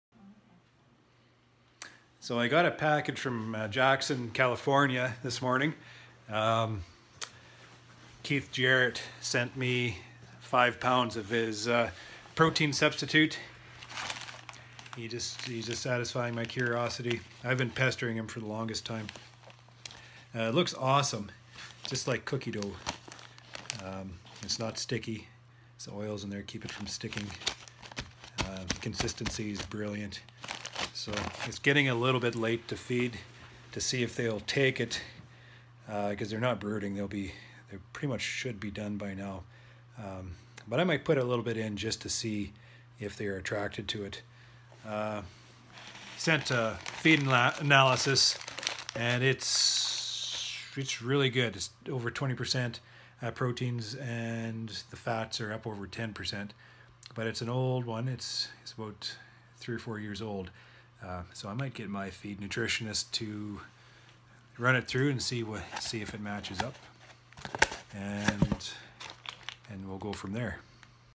a Canadian Beekeeper’s Blog - received a mail in sample of Nutrabee
Commercial Beekeeper Blog - October 3 2017
I received a sample of Nutrabee in the mail today.  I love it’s texture and consistency .